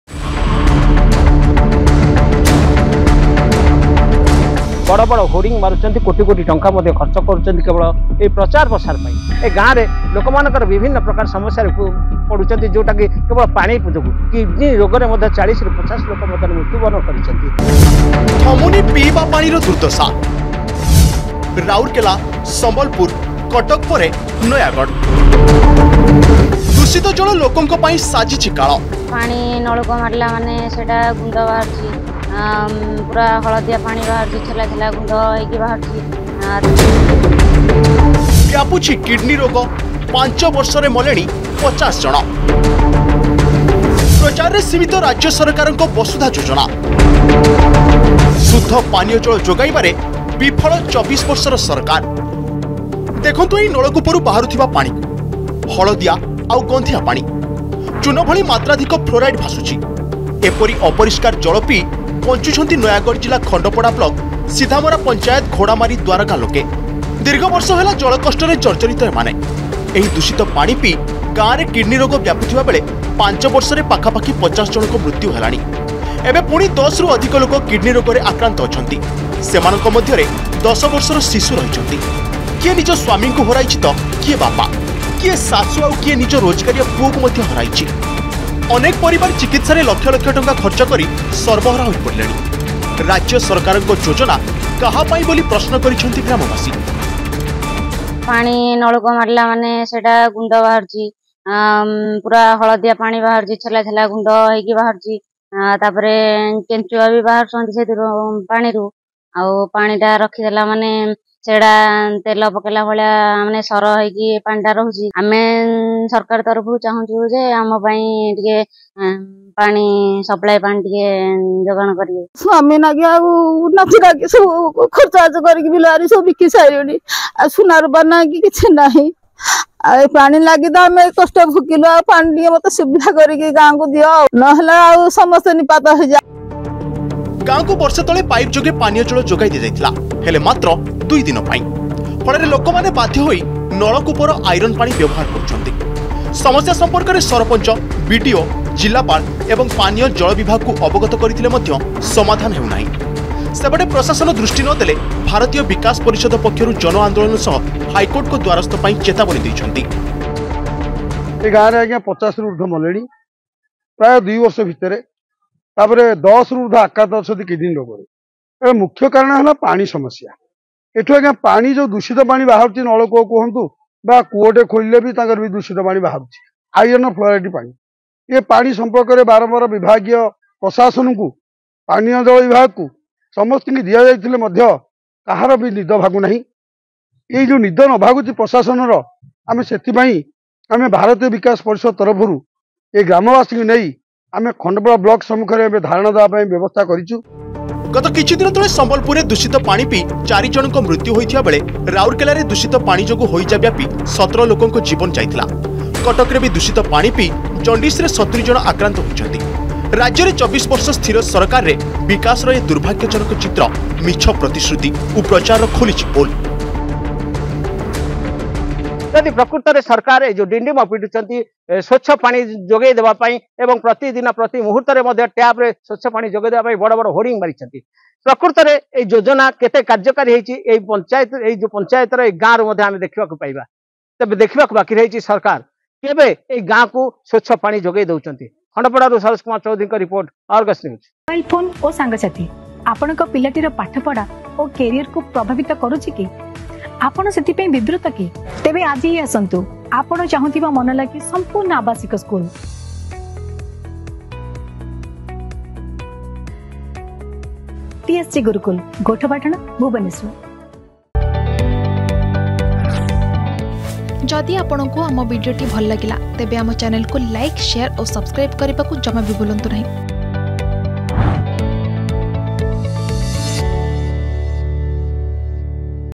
ଥମୁନି ପିଇବା ପାଣିର ଦୁର୍ଦ୍ଦଶା || Water Pollution
24 ବର୍ଷର ବିଜେଡି ସରକାରରେ ସରୁନି ଜଳ ଦୁର୍ଦ୍ଦଶା । ରାଉରକେଲା, ସମ୍ବଲପୁର, କଟକ ପରେ ନୟାଗଡ଼ରେ କାଳ ସାଜିଛି ଦୂଷିତ ଜଳ । କିଡନୀ ରୋଗରେ 5 ବର୍ଷରେ ଗଲେଣି 50 । ଫ୍ଲୋରାଇଡ୍ ପାଣି ନେଉଛି ଜୀବନ । ପ୍ରଚାରରେ ସୀମିତ ରାଜ୍ୟ ସରକାରଙ୍କ ବସୁଧା ଯୋଜନା । ଫିକା ପଡ଼ିଲା ପାନୀୟ ଜଳ ଯୋଗାଣର ମିଛ ଗୋଲାପି ଚିତ୍ର ।